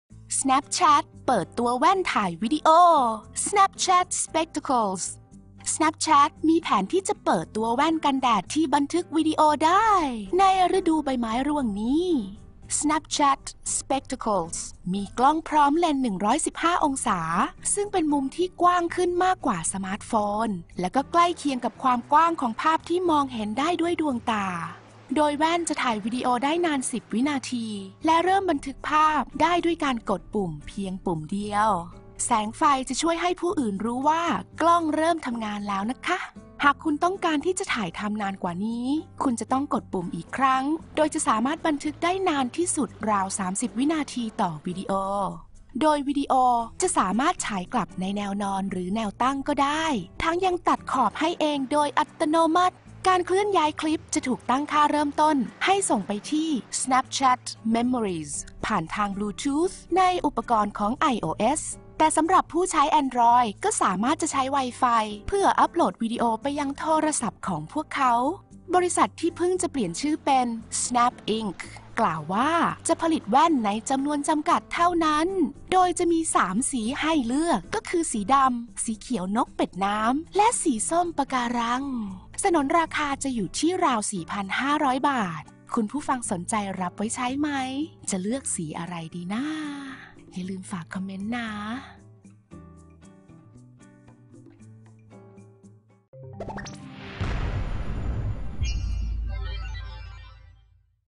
SNAPCHAT เปิดตัวแว่นถ่ายวิดิโอ Snapchat Spectacles
LOS ANGELES — Snapchat มีแผนที่จะเปิดตัวแว่นกันแดดที่บันทึกวิดีโอได้ ฤดูใบไม้ร่วงนี้

Snapchat Spectacles มีกล้องพร้อมเลนส์ 115 องศา ซึ่งเป็นมุมที่กว้างขึ้นกว่าสมาร์ทโฟน และใกล้ยเคียงกับความกว้างของภาพที่มองเห็นด้วยดวงตา

โดยแว่นจะถ่ายวิดีโอแบบได้นาน 10 วินาที และเริ่มบันทึกภาพได้ด้วยการกดปุ่มเพียงปุ่มเดียว แสงไฟจะช่วยให้ผู้อื่นรู้ว่ากล้องเริ่มทำงานแล้ว

หากคุณต้องการที่จะถ่ายทำนานกว่านี้ คุณจะต้องกดปุ่มอีกครั้ง โดยจะสามารถบันทึกได้นานสุดราว 30 วินาทีต่อวิดีโอ

วิดีโอที่สามารถฉายกลับในแนวนอนหรือแนวตั้งก็ได้ ทั้งยังตัดขอบอัตโนมัติ

การเคลื่อนย้ายคลิปจะถูกตั้งค่าเริ่มต้นให้ส่งไปที่ Snapchat Memories ผ่านทางบลูทูธ ในอุปกรณ์ iOS สำหรับผู้ใช้ Android ก็สามารถใช้ Wi-Fi เพื่ออัปโหลดวิดีโอไปยังโทรศัพท์ของพวกเขา

บริษัท ซึ่งเพิ่งเปลี่ยนชื่อเป็น Snap Inc กล่าวว่าจะผลิตแว่นในจำนวนจำกัด โดยจะมีสามสีให้เลือกคือสีดำ สีเขียวนกเป็ดน้ำ และสีส้มปะการัง สนนราคาอยู่ที่ราว 4,500 บาท